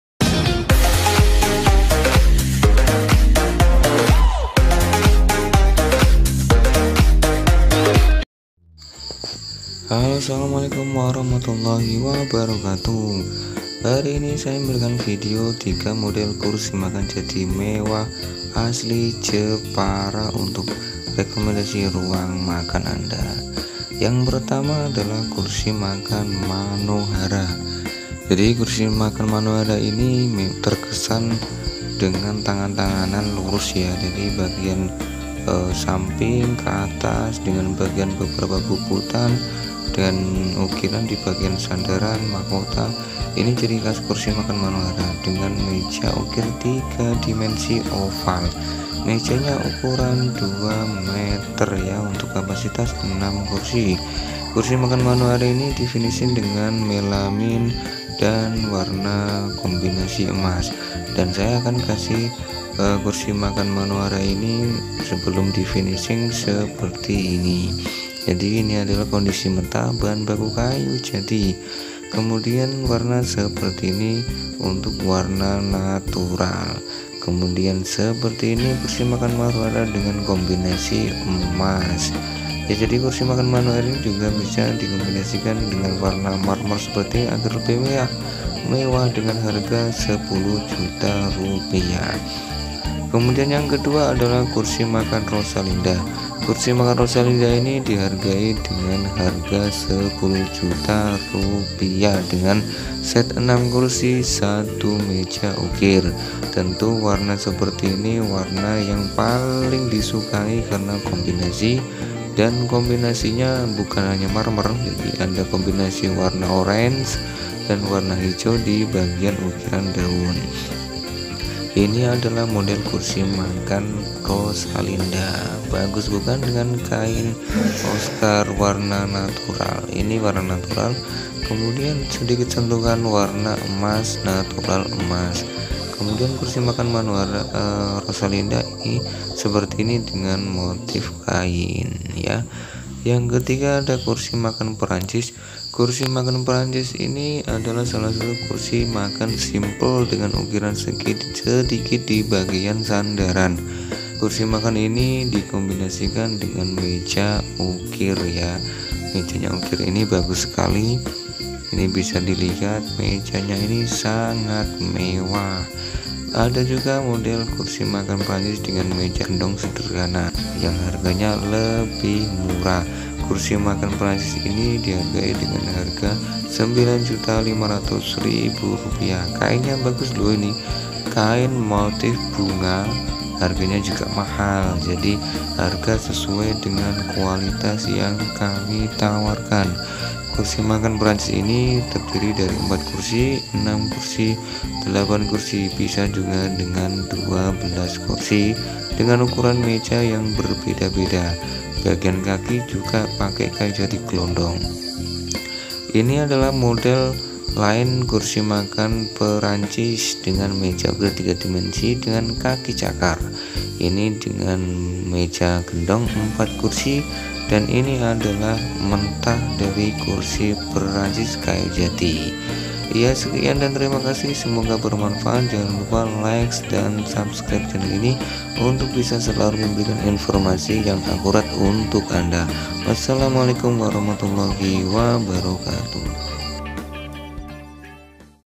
3 JENIS KURSI MAKAN MEWAH BUATAN JEPARA -Meja Makan Mewah
Kursi makan mewah berikut ini adalah kursi makan buatan asli dari jepara , bahan kayu jati dan warna finishing Motif Meja Marmer yang istimewa.